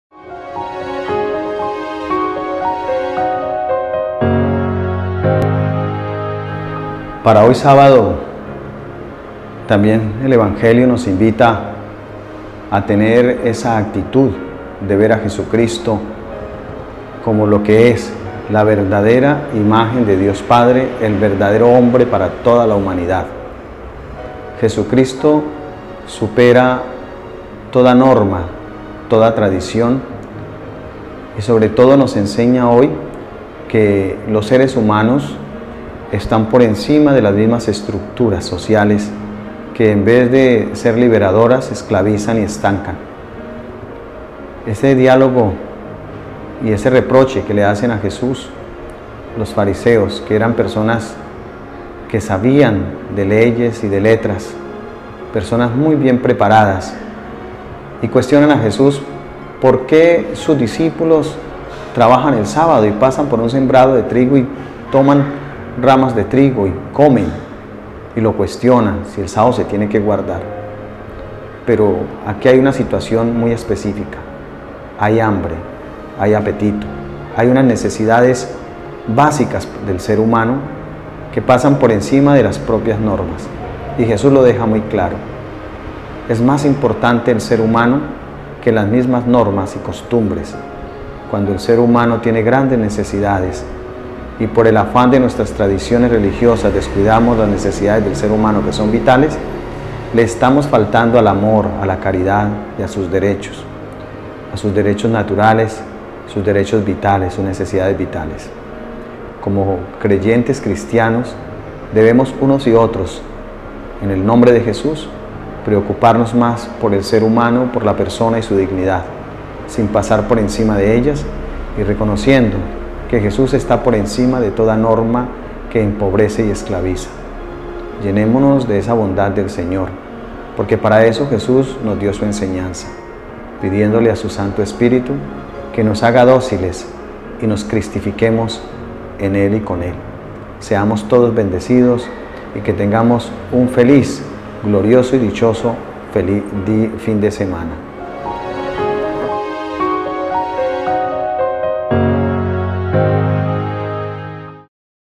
Palabras que son Luz - Sábado 07 de septiembre - Pbro. Duilver O. Galviz
EVANGELIO
¿Por qué hacen en sábado lo que no está permitido?

Un sábado, Jesús atravesaba un sembrado; sus discípulos arrancaban espigas y, frotándolas con las manos, se comían el grano. Unos fariseos les preguntaron: “¿Por qué hacen en sábado lo que no está permitido?”. Jesús les replicó: “¿No han leído lo que hizo David, cuando él y sus hombres sintieron hambre? Entró en la casa de Dios, tomó los panes presentados, que solo pueden comer los sacerdotes, comió él y les dio a sus compañeros”. Y añadió: “El Hijo del hombre es señor del sábado”.